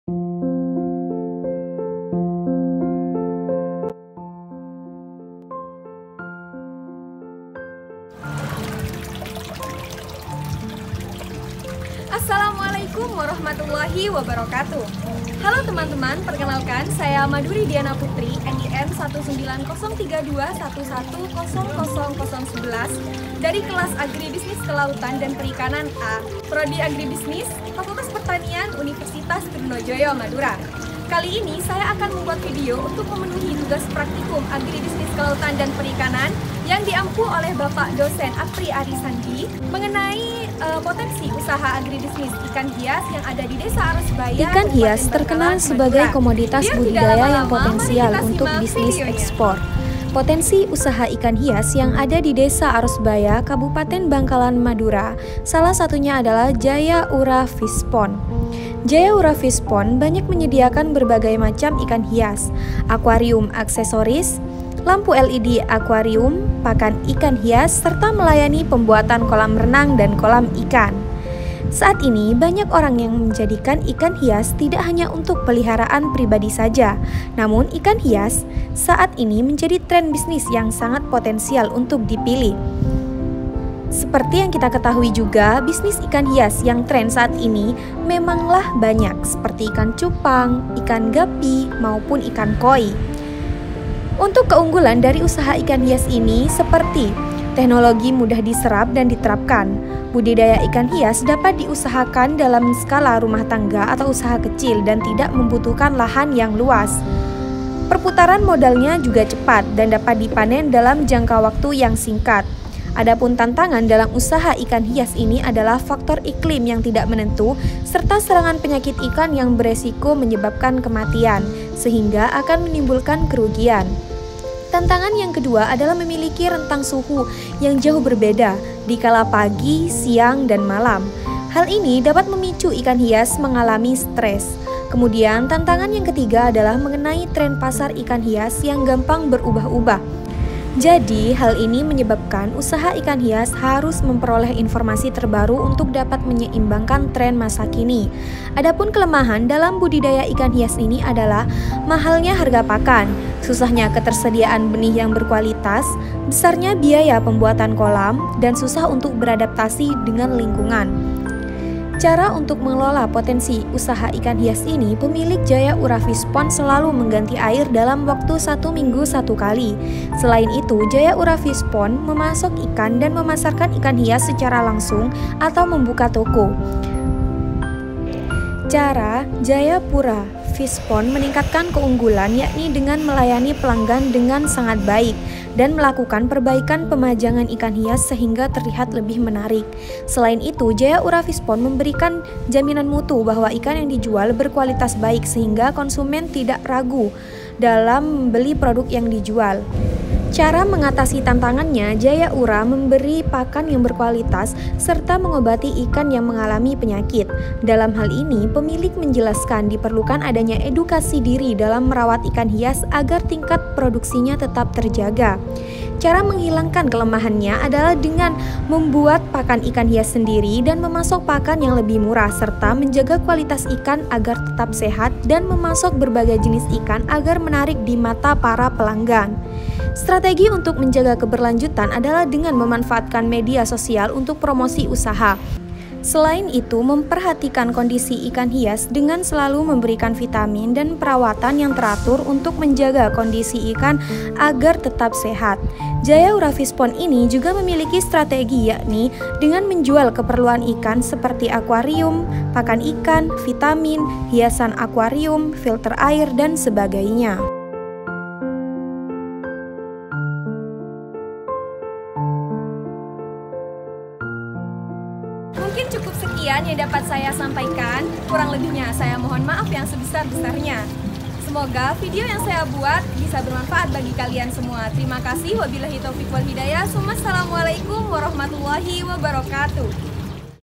Potensi Agribisnis Perikanan Budidaya Ikan Hias di Desa Arosbaya Bangkalan Madura
JuiNama : Maduri Diana Putri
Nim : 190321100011
Kelas : AKP (A)

Ikan hias terkenal sebagai komoditas budidaya yang potensial untuk bisnis ekspor.
Potensi usaha ikan hias yang ada di Desa Arosbaya Kabupaten Bangkalan Madura adalah Jayapura fish pond. Jayaura fish pond banyak menyediakan berbagai macam ikan hias, akuarium aksesoris, lampu LED aquarium, pakan ikan hias serta melayani pembuatan kolam renang dan kolam ikan.

Sedangkan untuk keunggulan dari usaha ikan hias ini adalah seperti

1. Teknologinya mudah diserap dan diterapkan, karena teknologi yang digunakan cukup sederhana.
2. Budi daya Ikan Hias Dapat diusahakan skala rumah tangga/ usaha kecil, tidak membutuhkan lahan yang luas.
3. Perputaran modal cepat, dapat dipanen dalam jangka waktu yang singkat. Budi daya ikan Hias mampu Menyerap tenaga kerja.

Adapun tantangan dalam usaha ikan hias ini adalah
Faktor iklim yang tidak menentu serta serangan penyakit ikan yang berisiko menyebabkan kematian ikan sehingga akan menimbulkan kerugian. tantangan kedua adalah memiliki rentang suhu yang jauh berbeda di kala pagi, siang, dan malam. Hal ini dapat memicu ikan hias mengalami stres.
Kemudian tantangan ketiga adalah mengenai tren pasar ikan hias yang gampang berubah. Jadi hal ini menyebabkan usaha ikan hias harus memperoleh informasi terbaru untuk dapat menyeimbangkan trend masa kini.

Adapun kelemahan dalam budidaya Ikan Hias ini adalah :
1.  Mahalnya harga pakan.
2.  Susahnya Ketersedian benih yang berkualitas.
3.  Besarnya biaya pembuatan kolam.
4.  Waktu pengontrolan setiap saat.
5.  Susah untuk beradaptasi dengan lingkungan.

Cara untuk mengelola potensi usahanya, pemilik JAyaura fish pond selalu mengganti air dalam waktu satu minggu satu kali. Selain itu JAyaura fish pond  memasok ikan dan memasarkan ikan hias secara langsung / membuka toko.
Cara Jayaura fish pond meningkatkan keunggulan yakni dengan melayani pelanggan dengan yang sangat baik. Dan melakukan perbaikan pemajangan ikan hias sehingga terlihat lebih menarik. Selain itu JAyaura fish pond memberi jaminan mutu bahwa ikan yang dijual berkualitas baik sehingga konsumen tidak ragu untuk membeli produk yang dijual dan hal ini akan memberi keuntungan bagi pemilik toko karena konsumen tersebut akan menjadi pelanggan.
Cara mengatasi tantangannya, Jayaura fish pond memberi pakan yang berkualitas serta mengobati ikan yang mengalami penyakit. Dalam hal ini, pemilik menjelaskan diperlukan adanya edukasi diri dalam merawat ikan hias agar tingkat produksinya tetap terjaga.
Cara menghilangkan kelemahan adalah adalah membuat pakan ikan hias sendiri, memasok pakan yang lebih murah serta menjaga kualitas ikan agar tetap sehat dan memasok berbagai jenis ikan agar menarik di mata para pelanggan.
Selain itu memperhatikan kondisi ikan hias  degan selalu memberikan vitamin dan perawatan yang teratur untuk menjaga kondisi ikan anda tetap sehat. Jayaura fish pond ini juga memiliki strategi yakni menjual keperluan ikan seperti aquarium, pakan ikan, vitamin, hiasan aquarium, filter air dan sebagainya.